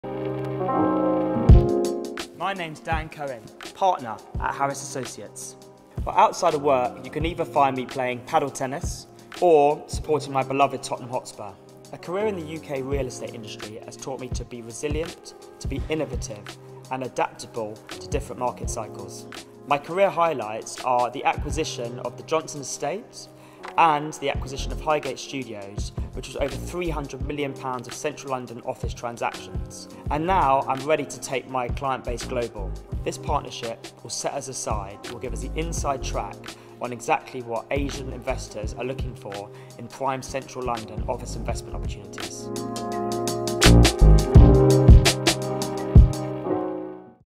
Dan Cohen, Partner at Harris Associates
Dan Cohen discusses the impact of global reach on the London Office market and how he can see investment opportunities through Harris Associates' strategic partnership with Edmund Tie.